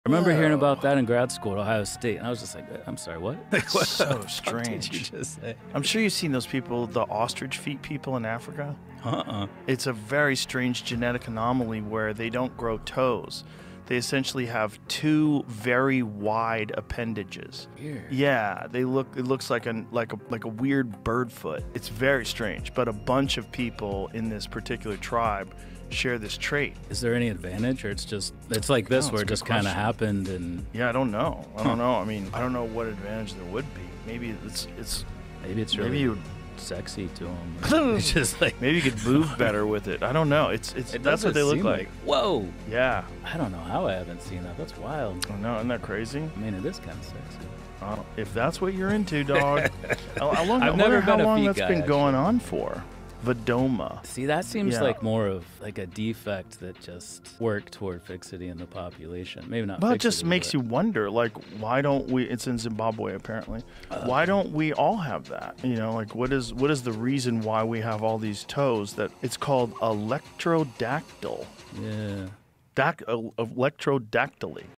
Bizarre 'Ostrich Feet' Genetic Anomaly Explained!
What if humans had bird feet? Meet the Vadoma tribe of Zimbabwe and their electodactyly genetic trait. Follow AngelX AI for more smart automation insights.